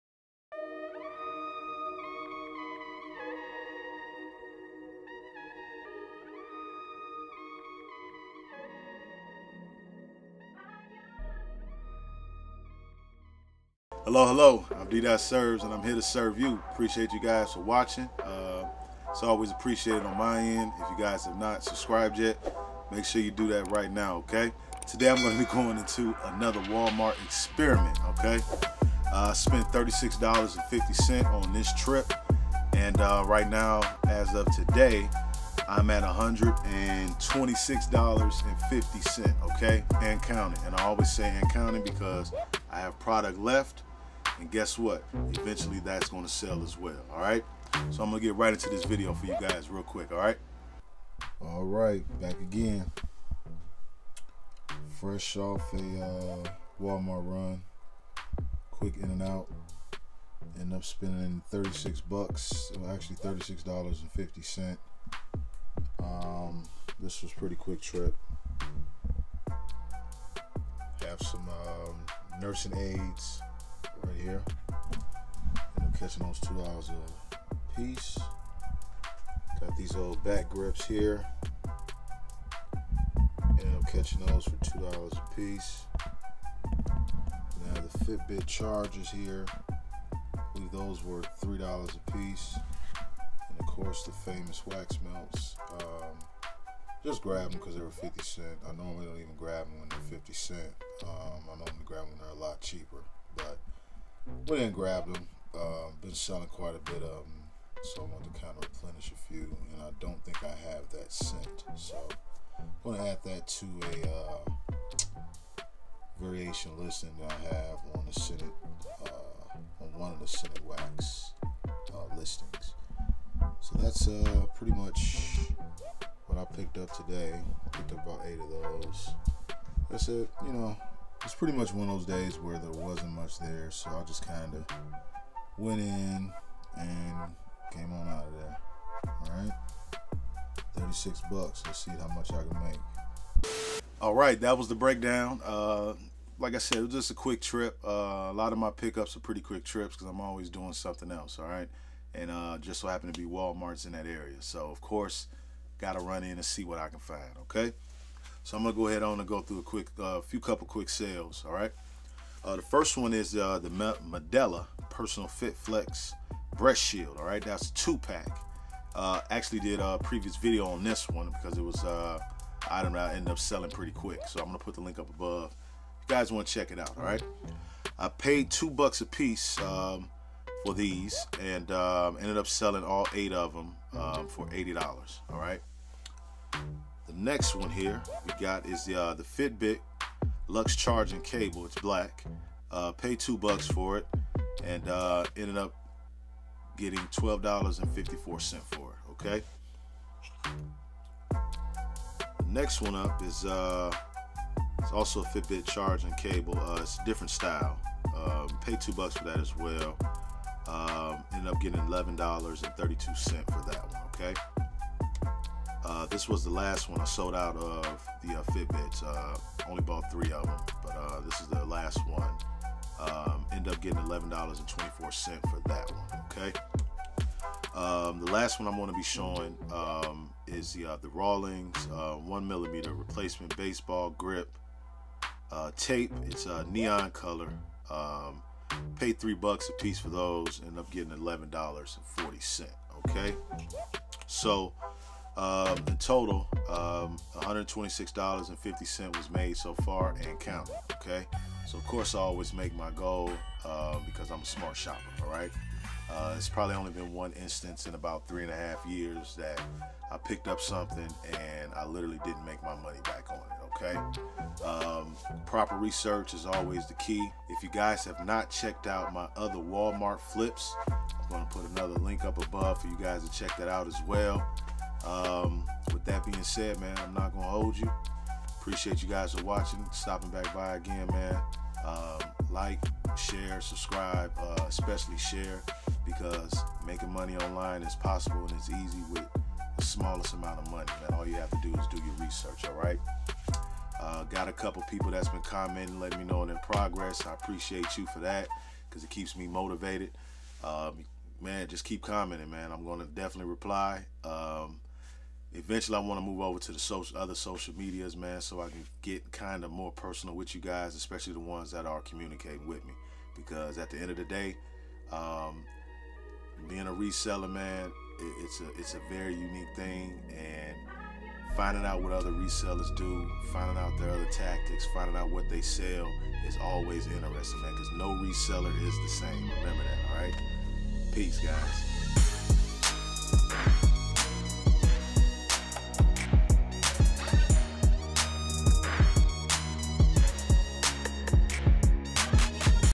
Quick $126.50 from shopping @ Walmart | Walmart Experiment | What Sold On eBay
In this episode D Dot breaks down another Walmart trip! The aim is always proper research to make sure you make profits! Sourcing from Walmart can bring you profits! Visiting several locations on a regular can help build your store!